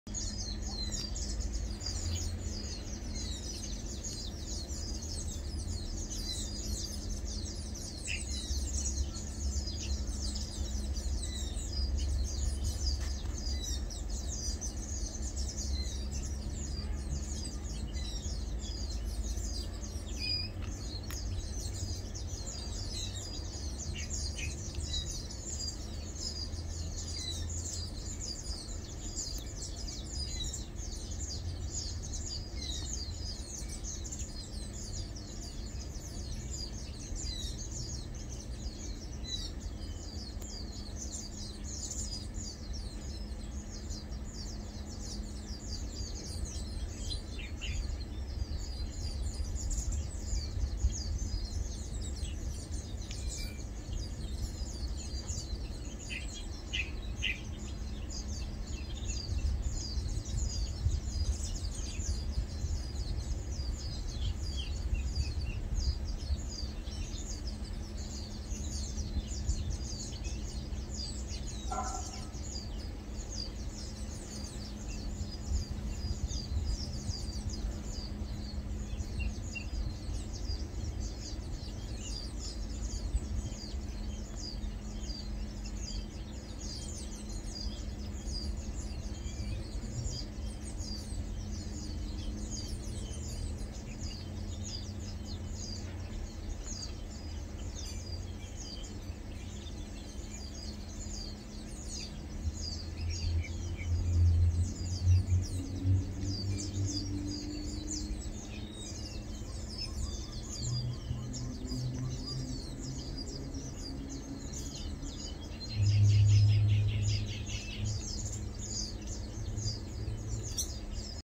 [03 Aug 2022] Yellow-vented Bulbul | Luklak (Pycnonotus goiavier) - Preening
The leg and wing stretch is so cute!

Spot the guest Zosterops meyeni!